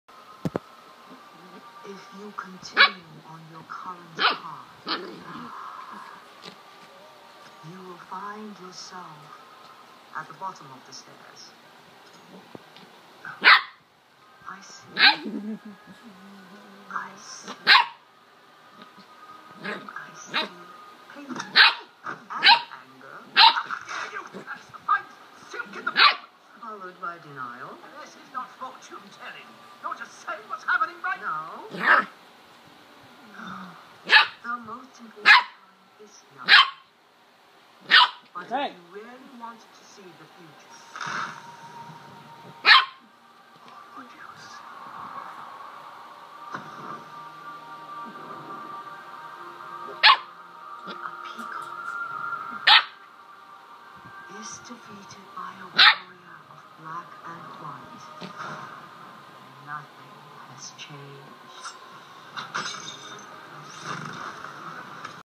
A corgi name Bella, playing in her bed all by herself.
She plays in her bed all by herself when she is bored. It is hilarious to watch!